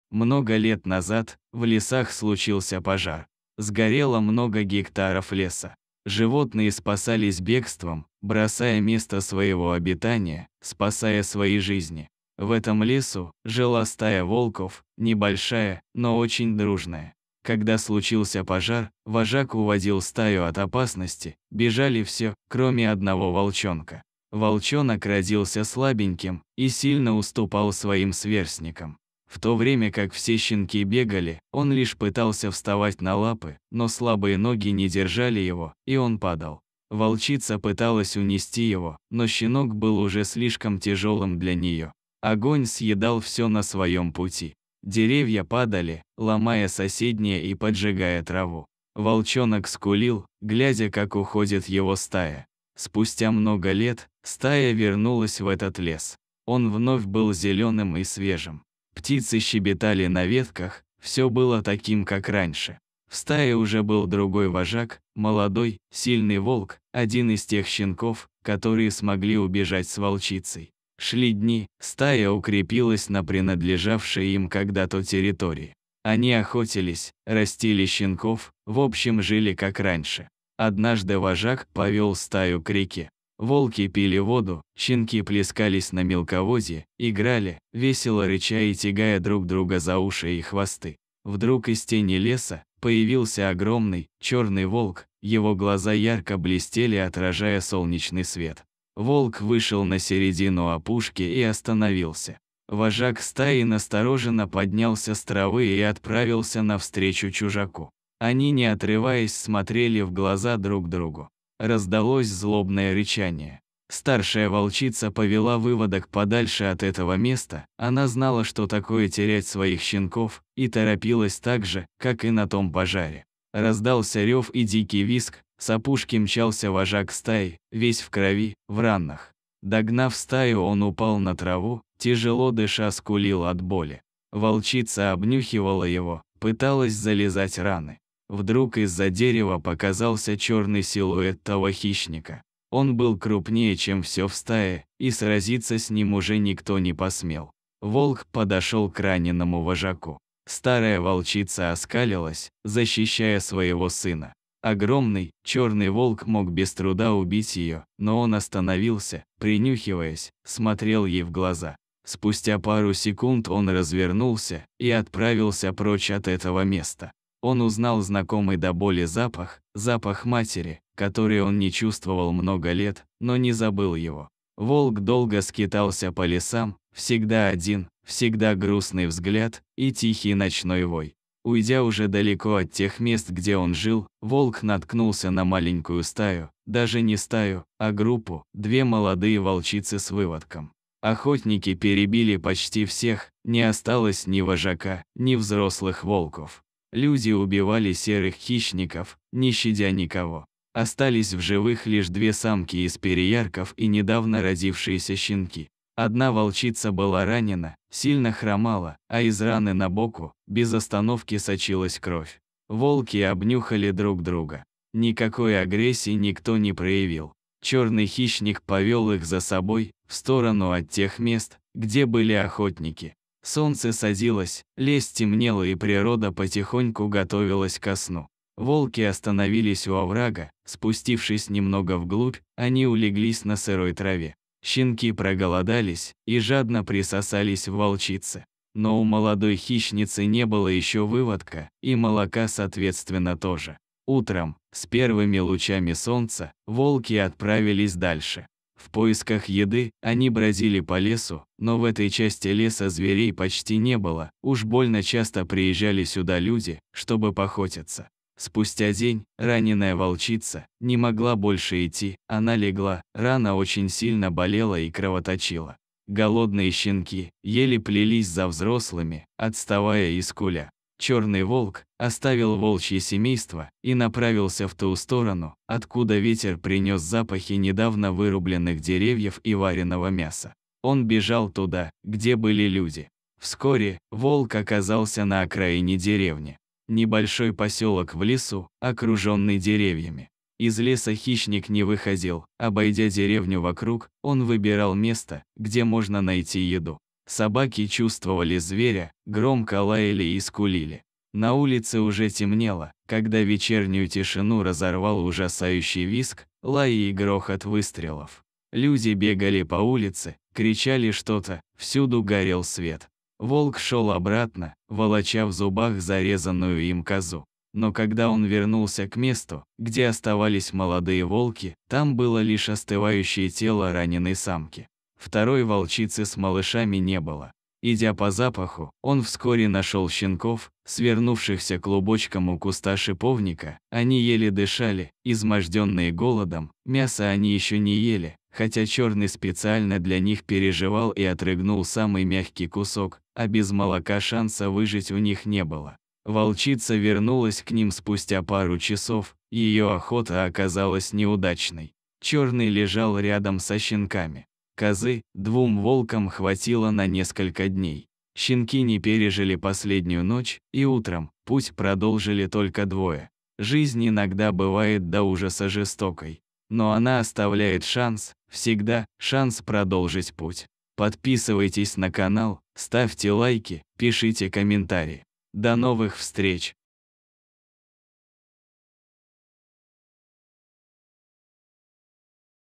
Волк узнал запах матери который не чувствовал несколько лет. Но он вынужден был уйти.
Много лет назад, в лесах случился пожар. Сгорело много гектаров леса. Животные спасались бегством, бросая место своего обитания, спасая свои жизни. В этом лесу, жила стая волков, небольшая, но очень дружная. Когда случился пожар, вожак уводил стаю от опасности, бежали все, кроме одного волчонка. Волчонок родился слабеньким, и сильно уступал своим сверстникам. В то время как все щенки бегали, он лишь пытался вставать на лапы, но слабые ноги не держали его, и он падал. Волчица пыталась унести его, но щенок был уже слишком тяжелым для нее... Огонь съедал всё на своем пути.